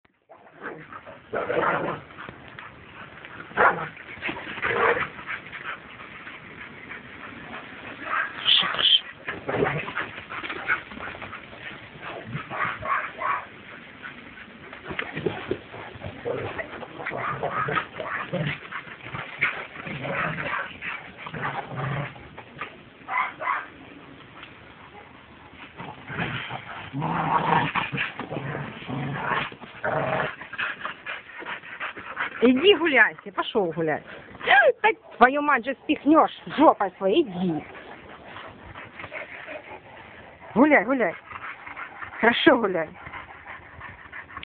Американский стаффтерьер против алабая, или Баха с Окташем бесятся :-)
Очередная игра американского стаффтерьера Бахиды и алабая Окташа.
10 сентября 2012 год.
«Дельта»/фантастика. Для подростков от 12 лет и взрослых.
 Платно:
 «От апортировки к проработке следа»/дрессировка собак.